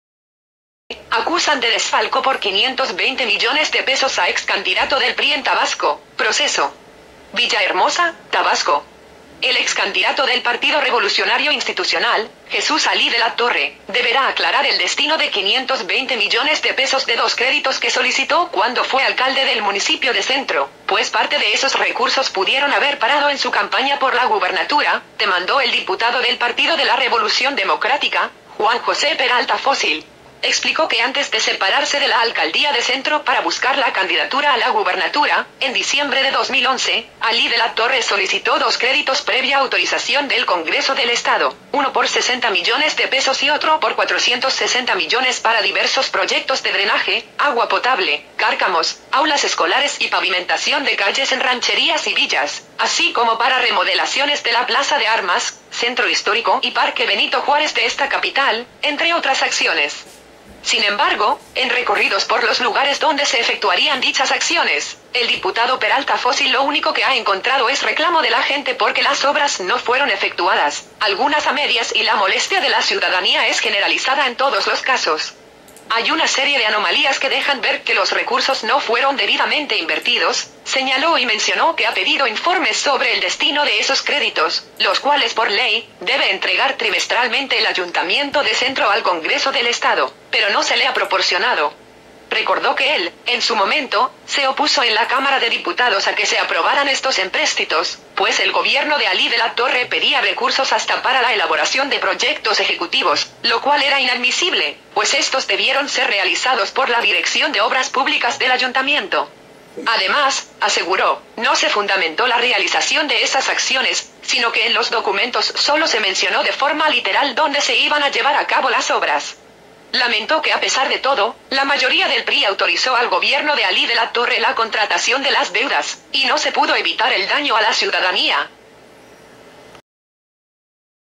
Acusan de desfalco por 520 MDP a ex candidato del PRI en Tabasco, PROCESO
Acusan de desfalco por 520 Millones De Pesos a ex candidato del PRI en Tabasco, PROCESO.

VILLAHERMOSA, Tabasco.- El ex candidato del Partido Revolucionario Institucional, Jesús Alí de la Torre, deberá aclarar el destino de 520 millones de pesos de dos créditos que solicitó cuando fue alcalde del municipio de Centro, pues parte de esos recursos pudieron haber parado en su campaña por la gubernatura, demandó el diputado del Partido de la Revolución Democrática, Juan José Peralta Fócil.
En entrevista concedida a un diario de importante circulación en el estado, explicó que antes de separarse de la alcaldía de Centro para buscar la candidatura a la gubernatura, en diciembre de 2 mil 11, Alí de la Torre solicitó dos créditos --previa autorización del Congreso del Estado-, uno por 60 millones de pesos y otro por 460 millones para diversos proyectos de drenaje, agua potable, cárcamos, aulas escolares y pavimentación de calles en rancherías y villas, así como para remodelaciones de la Plaza de Armas, Centro Histórico y parque Benito Juárez de esta capital, entre otras acciones.
Sin embargo, en recorridos por los lugares donde se efectuarían dichas acciones, el diputado Peralta Fócil lo único que ha encontrado es reclamo de la gente "porque las obras no fueron efectuadas, algunas a medias y la molestia de la ciudadanía es generalizada en todos los casos".
"Hay una serie de anomalías que dejan ver que los recursos no fueron debidamente invertidos", señaló y mencionó que ha pedido informes sobre el destino de esos créditos, los cuales por ley, debe entregar trimestralmente el ayuntamiento de Centro al Congreso del Estado, pero no se le ha proporcionado.
Recordó que él, en su momento, se opuso en la Cámara de Diputados a que se aprobaran estos empréstitos, pues el gobierno de Alí de la Torre pedía recursos "hasta para la elaboración de proyectos ejecutivos, lo cual era inadmisible, pues éstos debieron ser realizados por la Dirección de Obras Públicas del ayuntamiento".
Además, aseguró, no se fundamentó la realización de esas acciones, sino que en los documentos sólo se mencionó de forma "literal" donde se iban a llevar a cabo las obras.
Lamentó que a pesar de todo, la mayoría del PRI autorizó al gobierno de Alí de la Torre la contratación de las deudas, "y no se pudo evitar el daño a la ciudadanía".